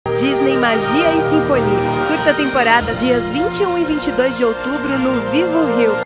Rio de Janeiro: Disney Magia e Sinfonia chegou 💙
O show que vai juntar todos os maiores universos da Disney em um espetáculo: Disney, Pixar, Marvel, Star Wars e National Geographic em perfeita harmonia! ✨

Acesse disneymagiaesinfonia.uhuu.com e confira mais informações 📲